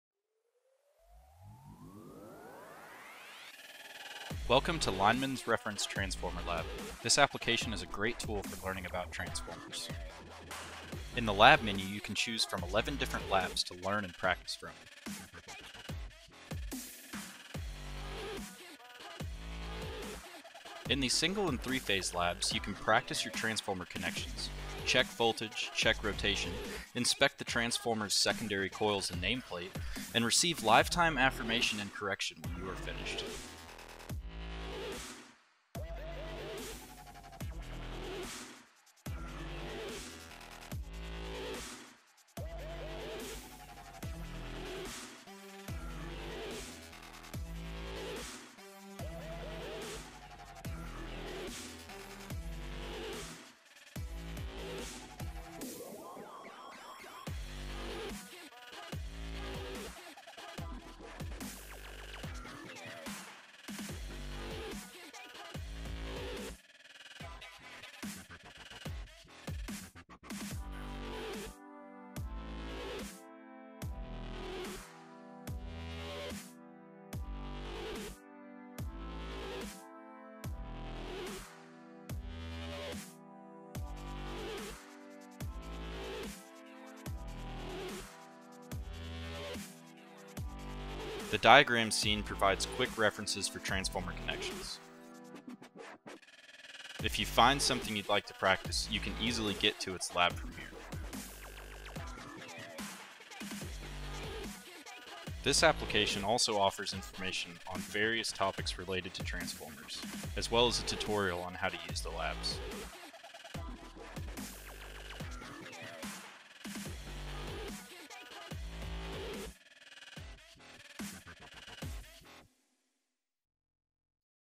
Lineman's Reference - XFMR Lab Promo Video
Fully interactive transformer lab for mobile. 11 labs to choose from and practice. This app gives you live affirmation and correction, something that used to only be achievable in the classroom or on the jobsite.

We do not claim any ownership to the music in this video.